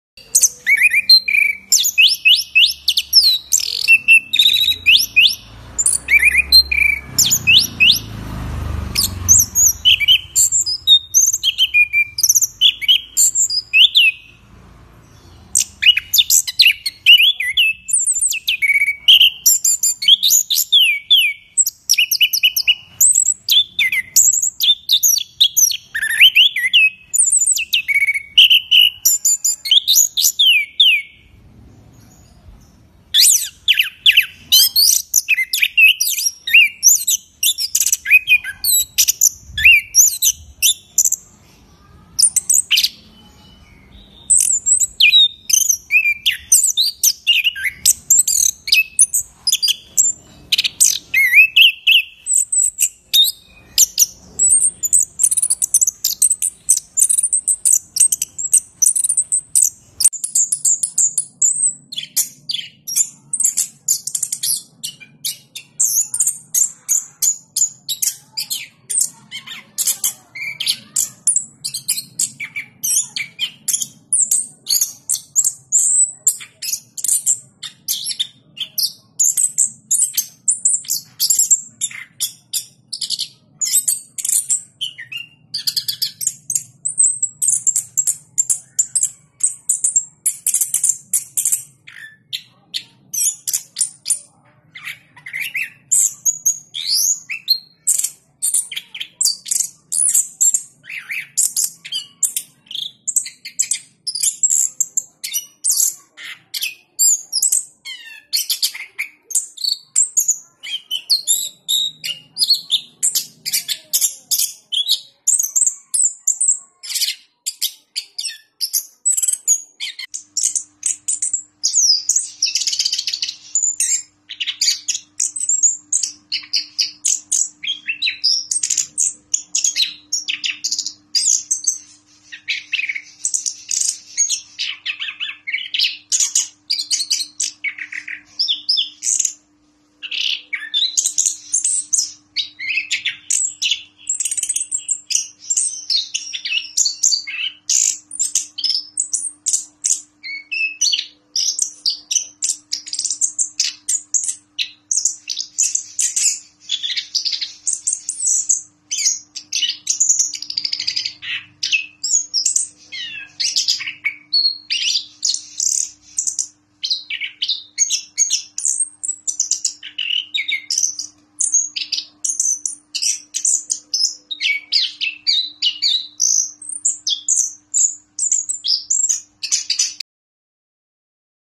Anis Kembang Gacor Ngeplong ISTIMEWA
Harga yg cukup fantasti bisa di bandrol pada burung ini, pernah menjuarai piala presiden di jogja.
Semoga bisa menjadi motivasi buat para pecinta anis kembang